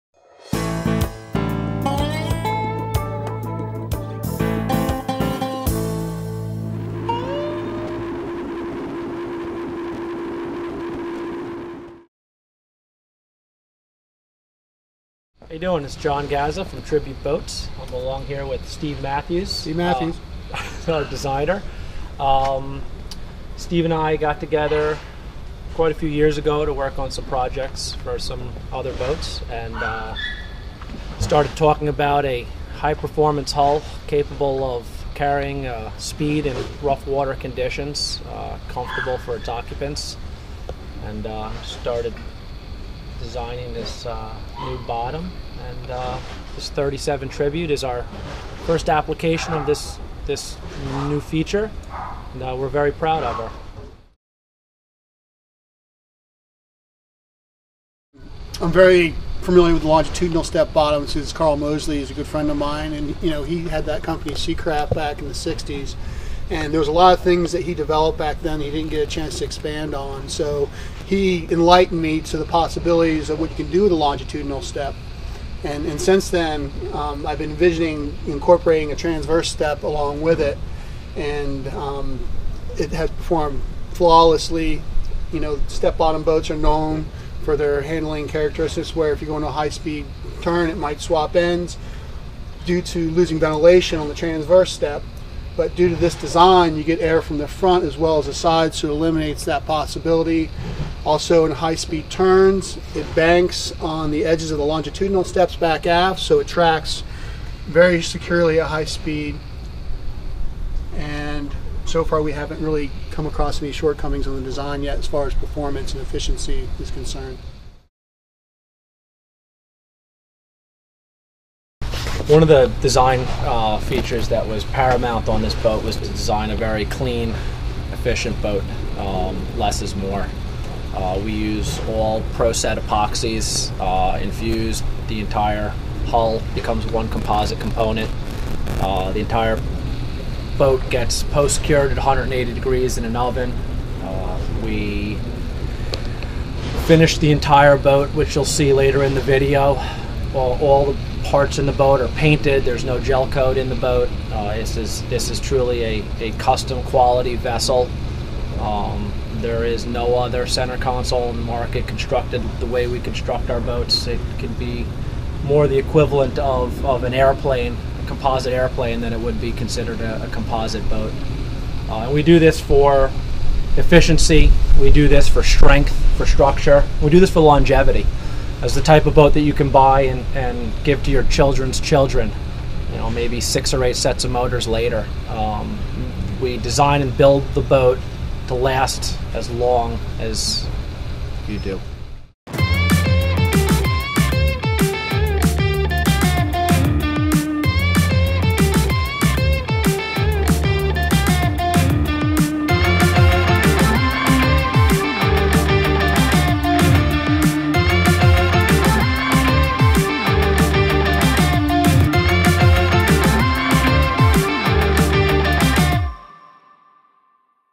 Tribute's 37 High Performance Center Console
Boat builder John Gaza and boat architect and designer Steve Matthews provide viewers with an in-depth look at the cutting edge step-hull bottom of the Tribute 37'. Why is the ride so good? Check out the first ever longitudinal and transverse Step-hull bottom. To see a complete walk-thru check out the next video titled "Tribute 37' From Stem to Stern.